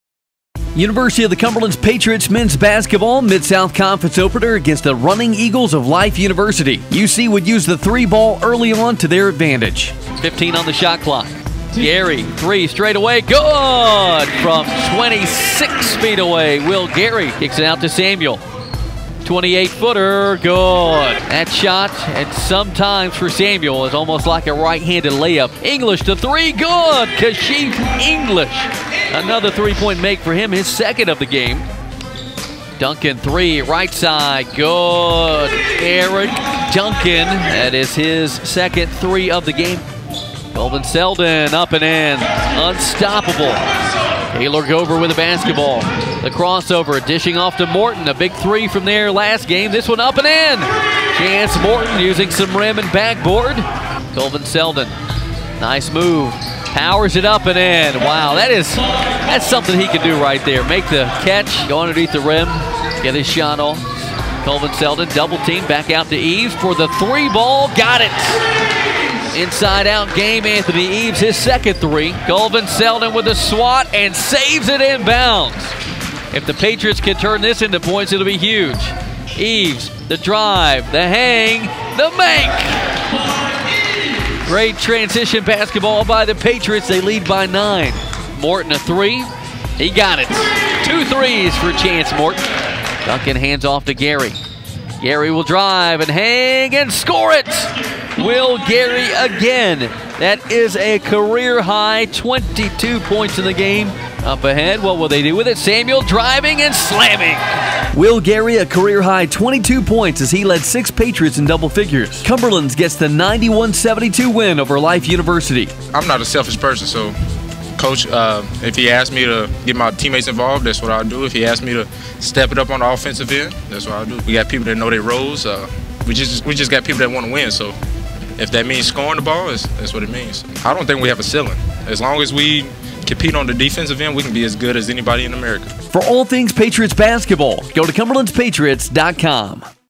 University of the Cumberlands - Men's Basketball vs. Life University 2017-2018
The University of the Cumberlands Patriots Men's Basketball team opened up Mid-South Conference play by defeating the Running Eagles of Life University (W 91-72) on January 6th, 2018 in Williamsburg, KY.